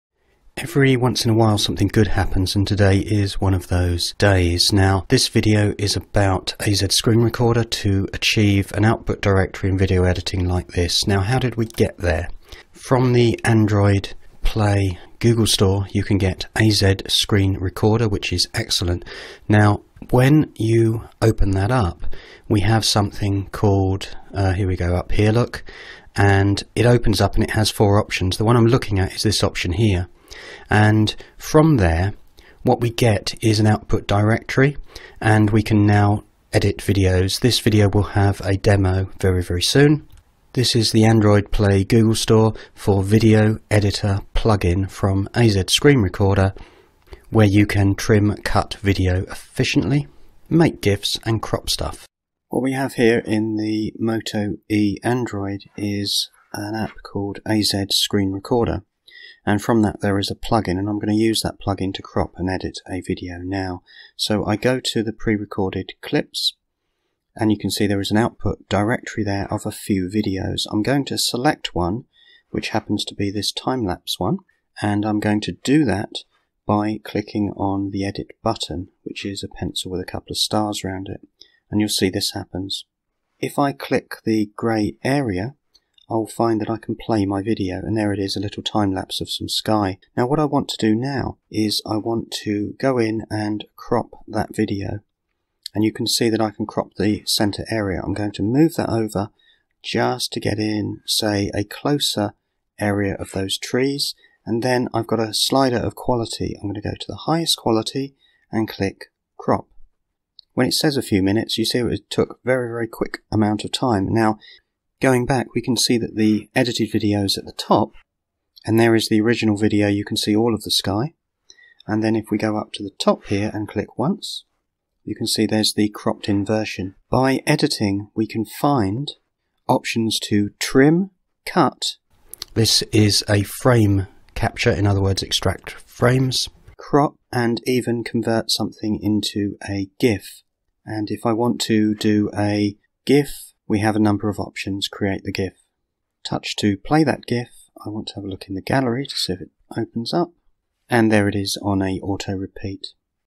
How to use the Android AZ Screen Recorder No Root new Plugin to edit crop videos
The AZ Screen Recorder is a brilliant App -- now it's even better with the new Plugin to edit Trim, Crop and gif ANY video. Step 1, record your android screen. Step 2 edit and Step 3 just copy ANY video to android AZ directory and edit as you wish..

passionately curious i make videos to help people learn . complex made simple fast . mike downes media